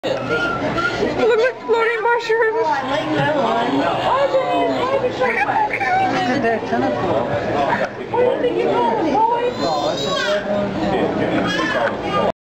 OMG!! I LOVE JELLIES!!
I went to the Baltimore Inner harbor Aquarium and saw the new Jellyfish exhibit! OMG MUSHROOM JELLIES! SOOOO CUTE. ^.^ i have a minor obsession with jellyfish and these are so stubby and adorable